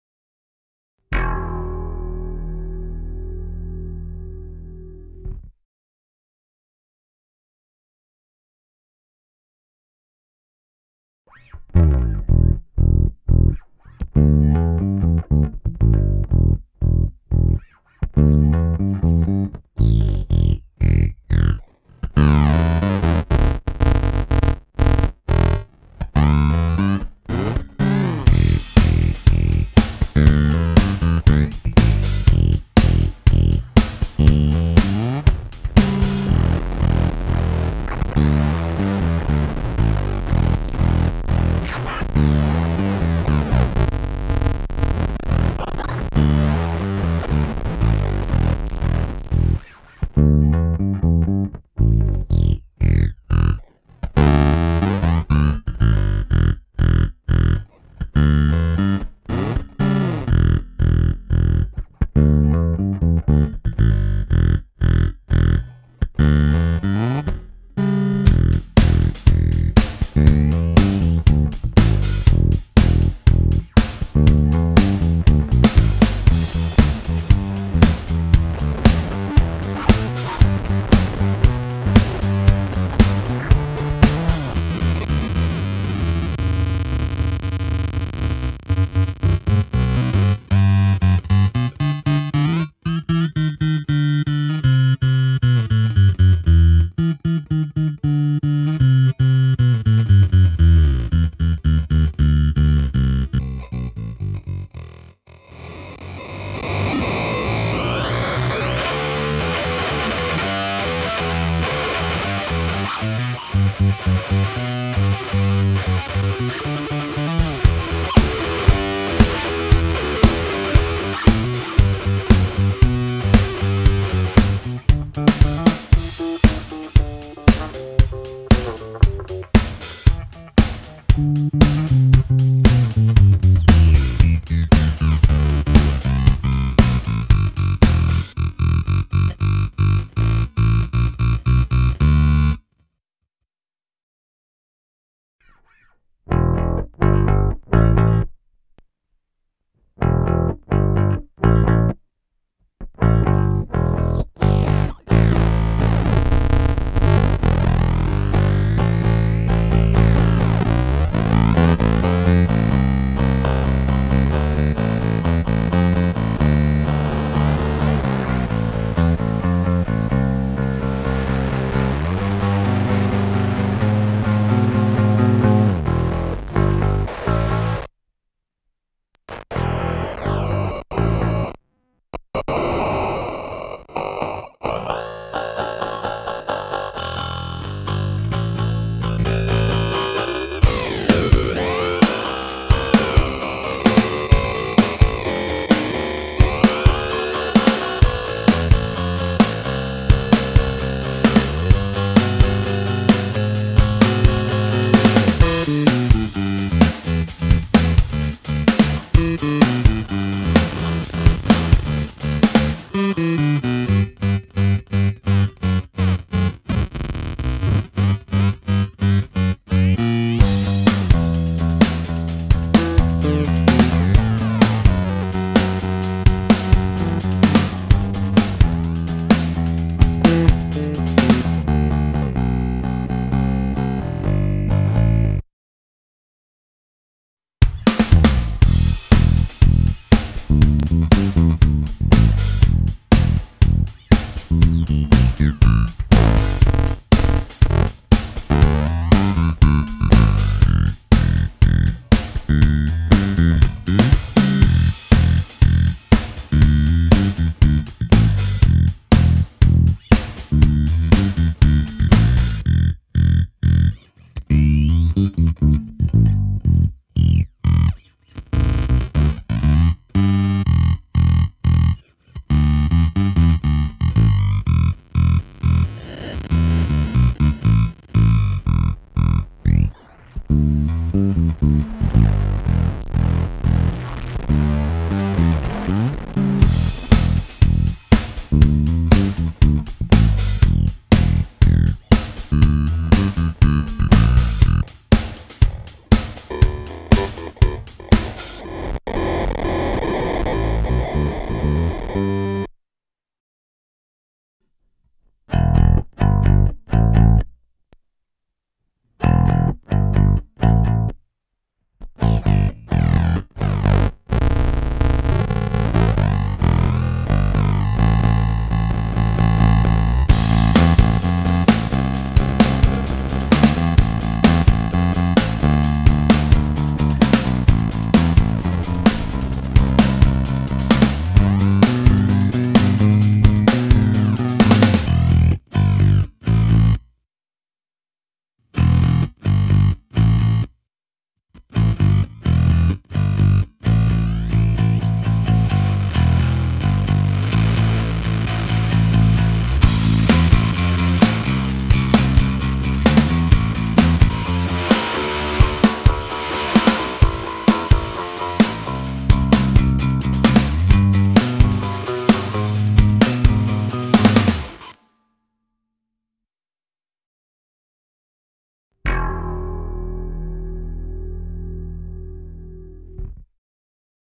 Hotone - Krush
Demo of the Krush by Hotone Audio

► Antelope Zen Studio out ► Radial JCR Reamper ► Pedal ► Antelope Zen Studio DI
*Brainworx - SVT-VR for amp simulation sections

Custom P Bass:

- 73 P Bass neck
- P Bass custom made alder body
- Aguilar PJ Pickups, just using the P
- Tone 100% open

Custom Jazz Bass:

- 82 Squier Bullet neck
- Allparts Jazz Bass Ash body
- Fender Custom shop '60s JJ Pickups, both full on
- Tone 100% open